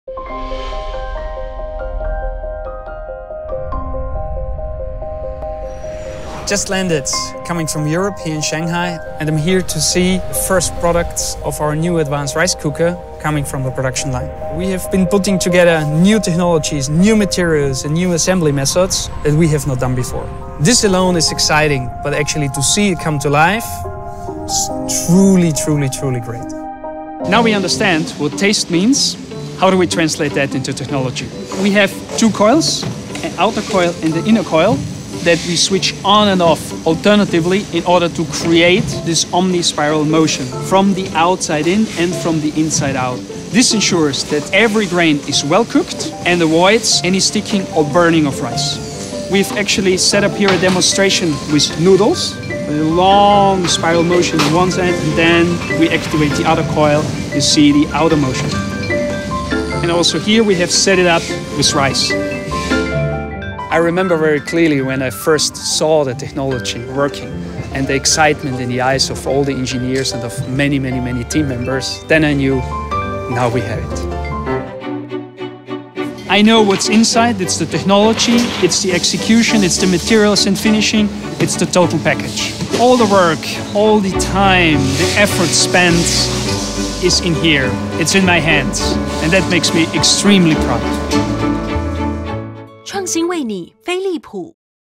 飛利浦雙向智旋IH電子鍋 - 雙向智旋技術說明
飛利浦雙向智旋IH電子鍋-轉速決定Q度

創新以晶片控制正反對流，讓米飯每分鐘翻滾300次*。
加熱均勻度領先63%，彈性更提高10.7%**，釋放每粒米的美味極限。

*轉速為米水比1:4實驗測得沸點時瞬間最高值，不代表全程，實際狀況因使用條件不同有所差距。
**加熱均勻度及彈性數據為飛利浦亞洲研究院實驗測得，對照基準為飛利浦非IH電子鍋。